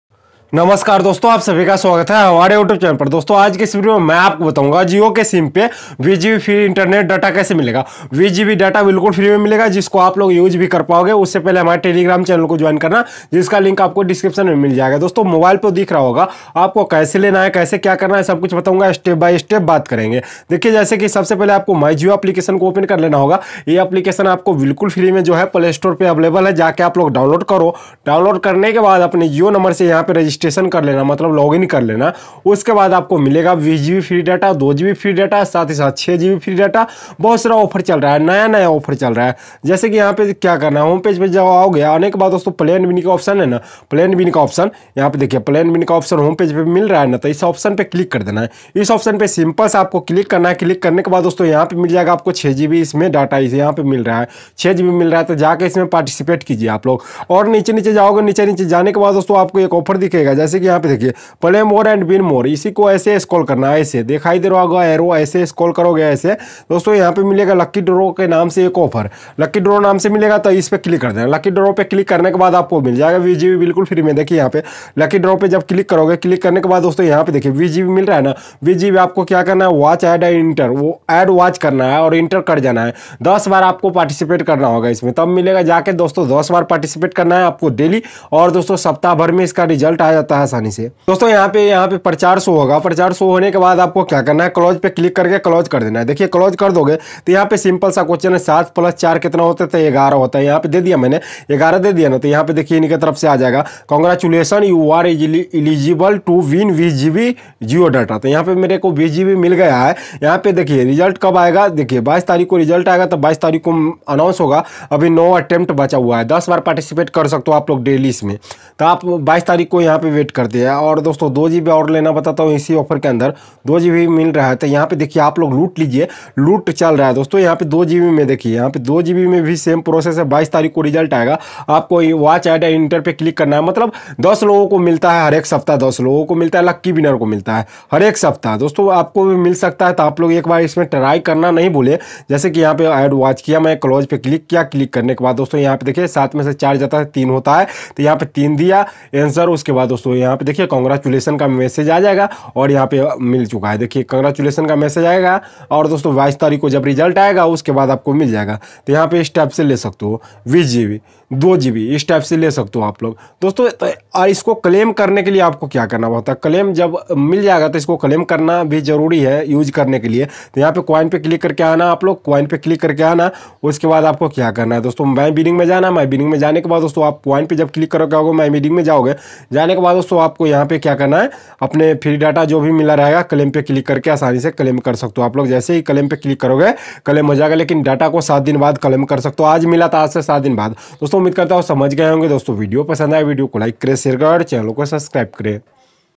Jio 20GB FREE Data Offer | Jio Free Data 2024 | Jio Free Internet Offer | Jio Free Data Claim Code
Dosto Aaj Ke Is Video Me Maine Aapko Bataya Hai Jio Sim Mein Free Data Kaise Milenge Iske Bare Me